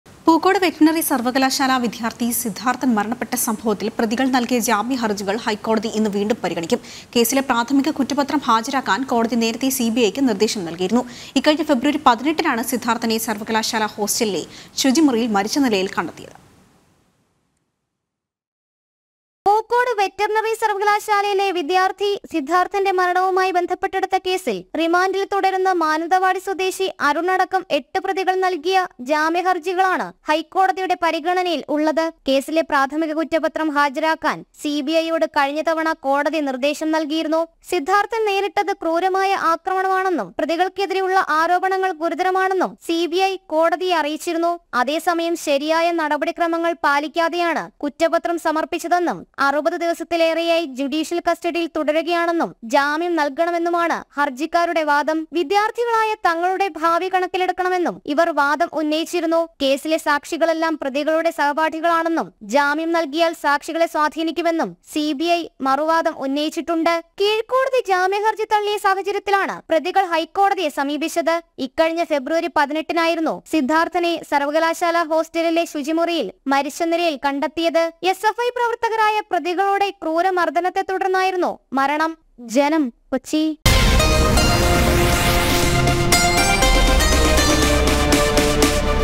സിദ്ധാർത്ഥന്റെ മരണത്തിൽ പ്രതികളുടെ ജാമ്യ ഹർജികൾ ഹൈക്കോടതി ഇന്ന് വീണ്ടും പരിഗണിക്കും | HIGH COURT
പൂക്കോട് വെറ്ററിനറി സർവകലാശാല വിദ്യാർഥി സിദ്ധാർത്ഥൻ മരണപ്പെട്ട സംഭവത്തിൽ പ്രതികൾ നൽകിയ ജാമ്യ ഹർജികൾ ഹൈക്കോടതി ഇന്ന് വീണ്ടും പരിഗണിക്കും. കേസിലെ പ്രാഥമിക കുറ്റപത്രം ഹാജരാക്കാൻ കോടതി നേരത്തെ CBIക്ക്  നിർദേശം നൽകിയിരുന്നു. ഇക്കഴിഞ്ഞ ഫെബ്രുവരി 18നാണ് സിദ്ധാർത്ഥനെ സർവകലാശാല ഹോസ്റ്റലിലെ ശുചിമുറിയിൽ മരിച്ച നിലയിൽ കണ്ടെത്തിയത്.

NEWS ANCHOR : ATHIRA SASI